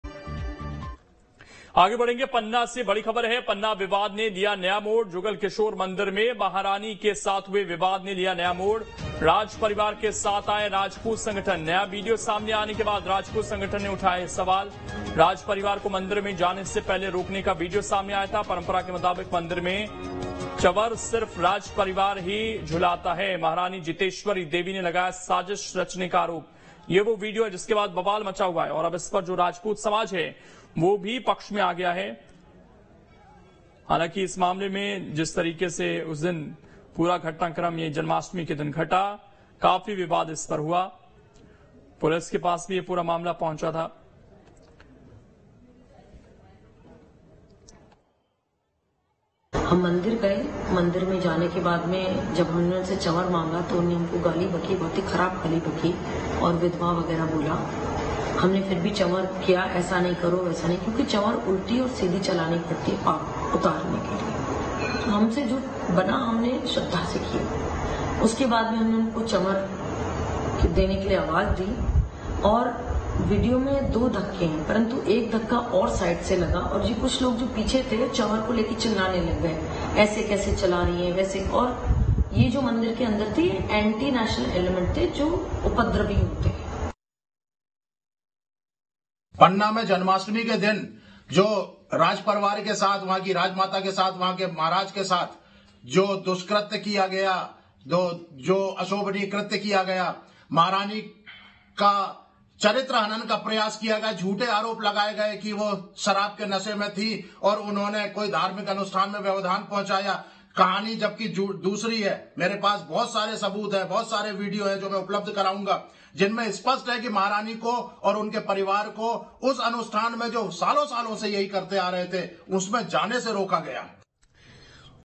Panna विवाद में आया नया मोड़, राजपरिवार के साथ आए ये लोग | Latest Update | News State MP-CG
पन्ना राजघराने की महारानी का बयान.. मंदिर प्रबंधन पर लगाए गंभीर आरोप.. महारानी ने गाली-गलौज करने का लगाया आरोप.. मेरे साथ अभद्र व्यवहार किया गया-जितेश्वरी देवी

Statement of the Queen of Panna royal family.. Serious allegations made against the temple management.. The Queen accused of abusing.. I was treated indecently - Jiteshwari Devi.